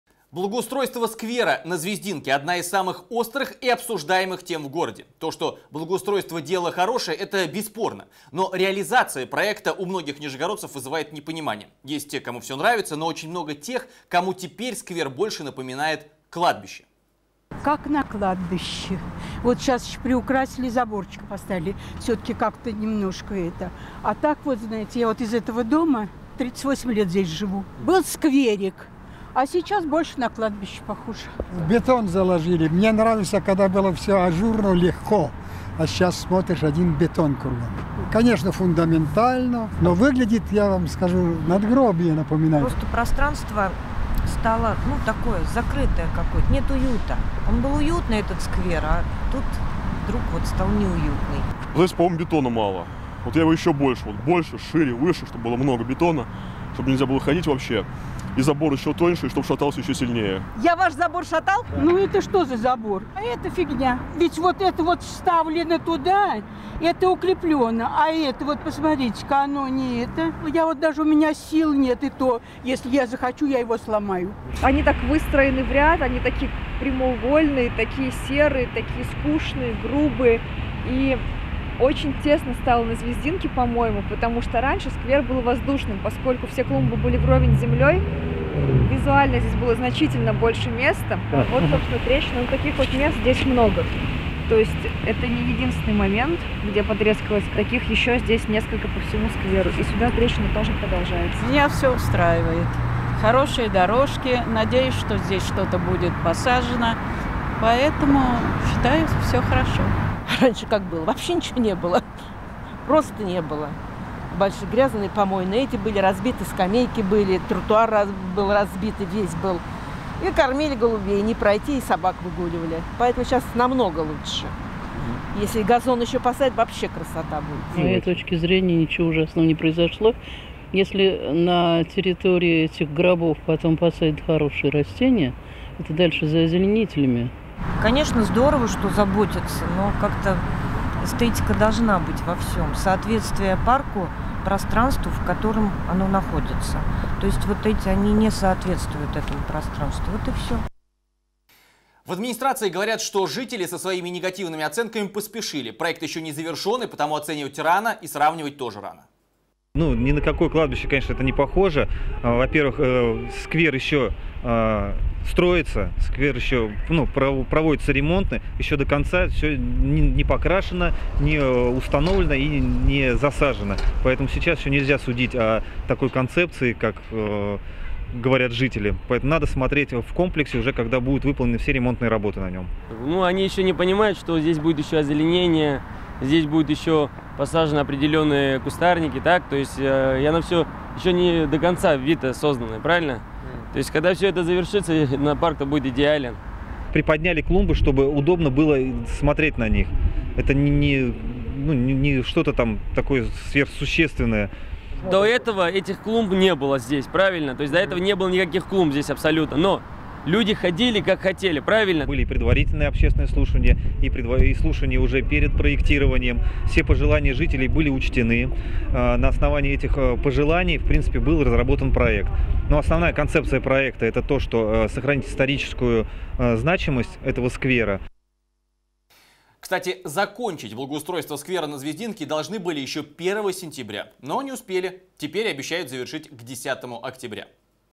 Спорное благоустройство - сквер на Звездинке стал одной из главных обсуждаемых тем
Благоустройство сквера на Звездинке сейчас одна из самых острых и обсуждаемых тем. То, что благоустройство дело хорошее - это не обсуждается, но реализация проекта на деле у многих нижегородцев вызывает непонимание. Есть те, кому все нравится, но очень много и тех - кому теперь сквер больше напоминает кладбище.